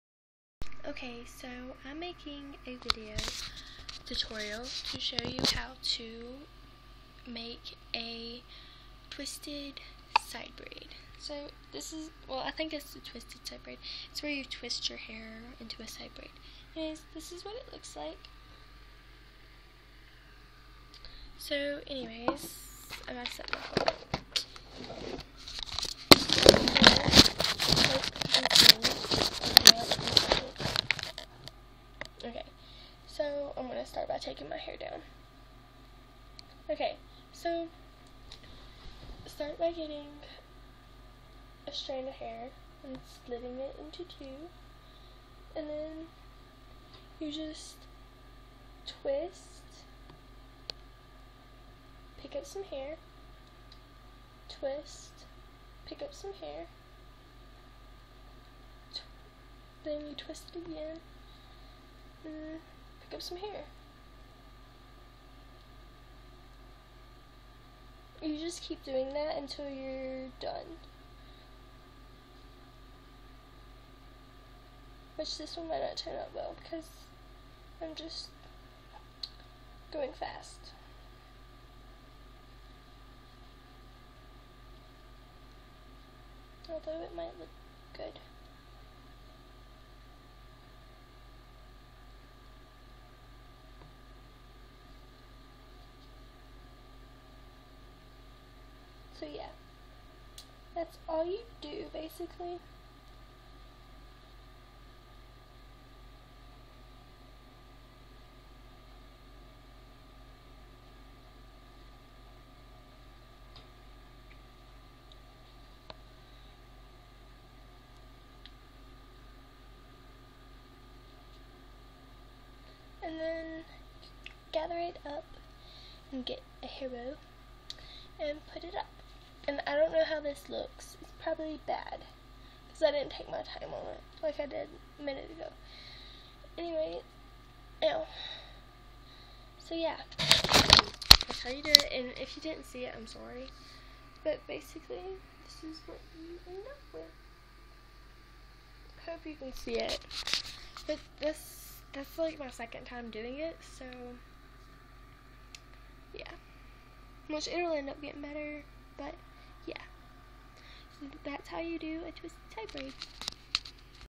Twisted side ponytail
I ment twisted side pony tail but u can also braid it into your haair if you want to but kept saying braid for some reason